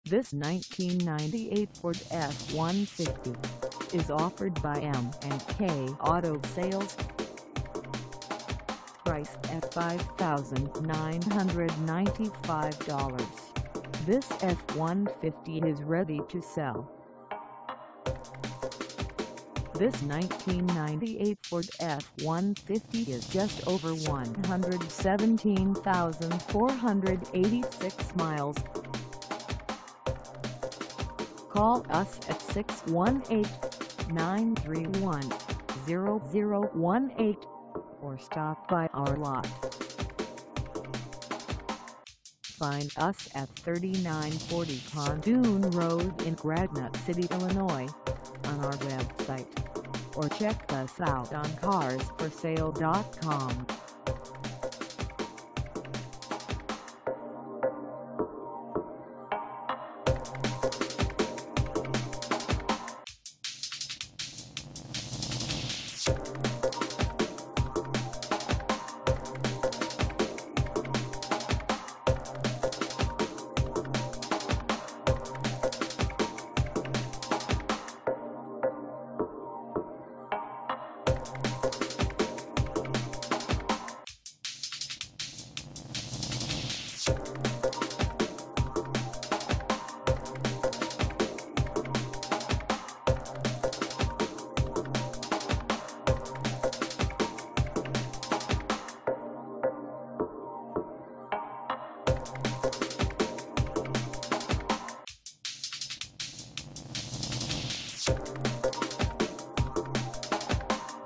1998 Ford F150 XLT SuperCab Short Bed 2WD - for sale in Gran
M and K Auto Sales
3940 Pontoon Rd
in Granite City, IL 62040

Come test drive this 1998 Ford F150 XLT SuperCab Short Bed 2WD for sale in Granite City, IL.

VERY NICE -LOW COST TRUCK, WON'T LAST LONG